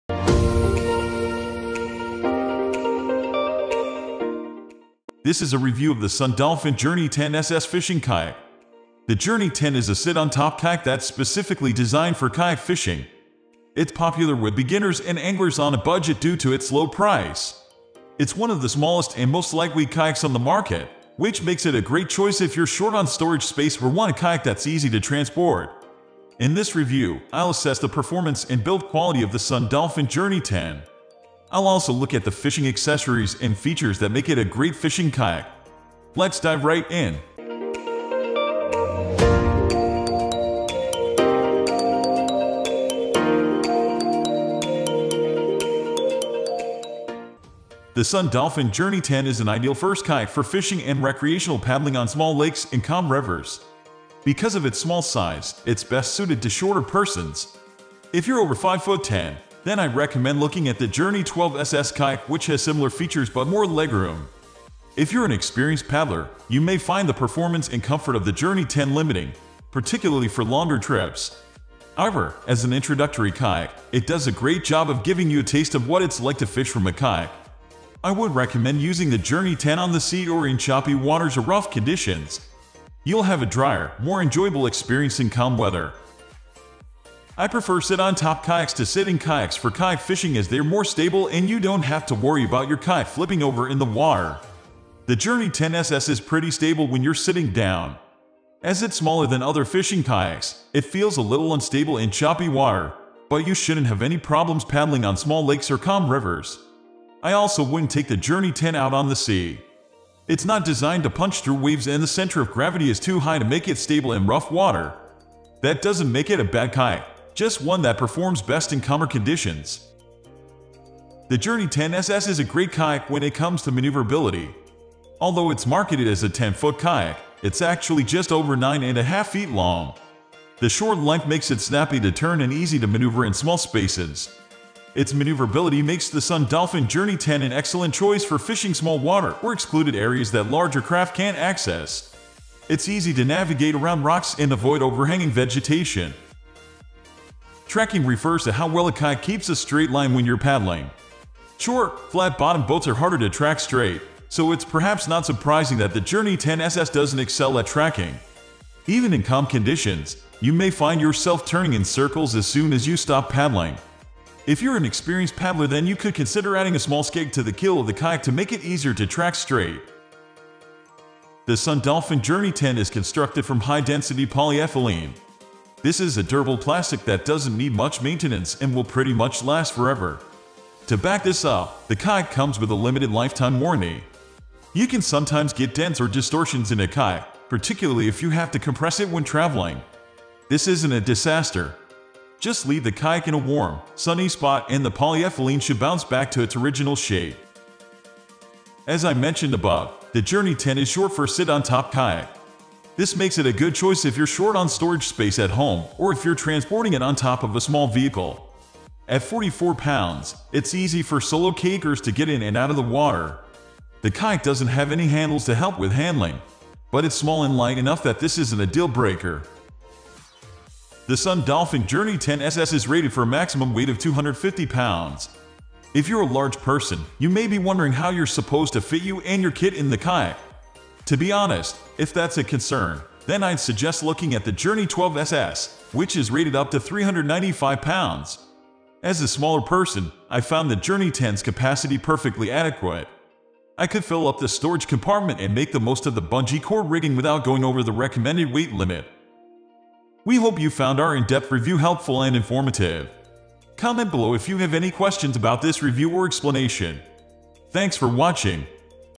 Sun Dolphin Journey 10 SS Fishing Kayak Review
The Journey 10 is a sit-on-top kayak that’s specifically designed for kayak fishing. It’s popular with beginners and anglers on a budget due to its low price.

It’s one of the smallest and most lightweight kayaks on the market, which makes it a great choice if you’re short on storage space or want a kayak that’s easy to transport.

In this review, I’ll assess the performance and build quality of the Sun Dolphin Journey 10. I’ll also look at the fishing accessories and features that make it a great fishing kayak.